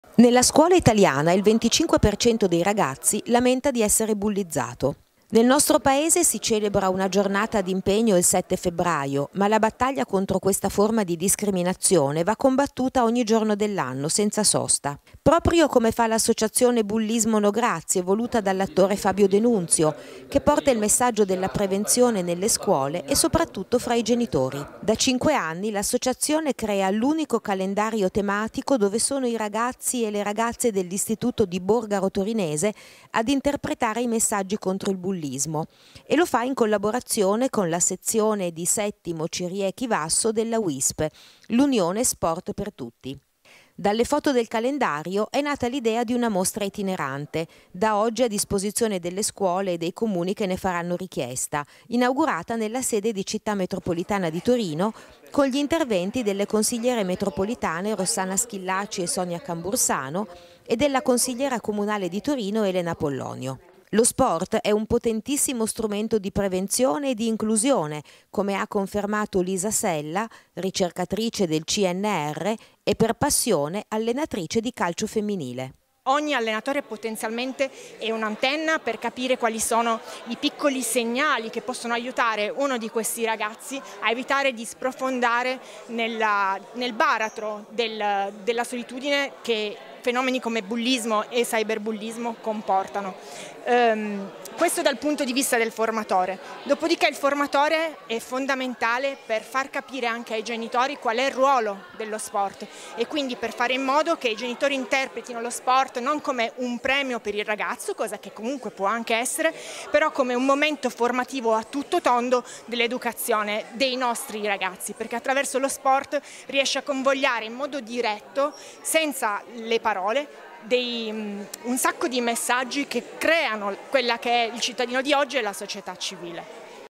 Combattere il bullismo
Questa mattina nella sede della Città metropolitana è stata presentata una mostra fotografica dell'Associazione" bullismo no grazie" fondata dall'attore Fabio de Nunzio, in collaborazione con la Uisp di Settimo, Ciriè Chivasso.
Nelle foto i ragazzi dell'istituto di Borgaro interpretano scene iconiche che hanno un forte messaggio contro il bullismo.
Intervista a Lisa Sella ricercatrice del Cnr e allenatrice di calcio